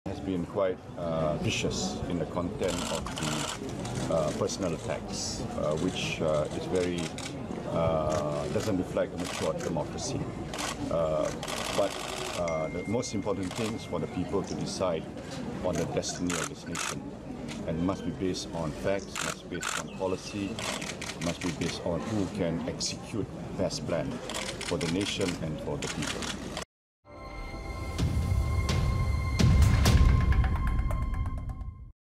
Malaysian PM Razak confident of election victory
Incumbent Malaysian Prime Minister Najib Razak said he was confident of victory following what he described as "quite vicious" personal attacks during the campaign, as he cast his vote.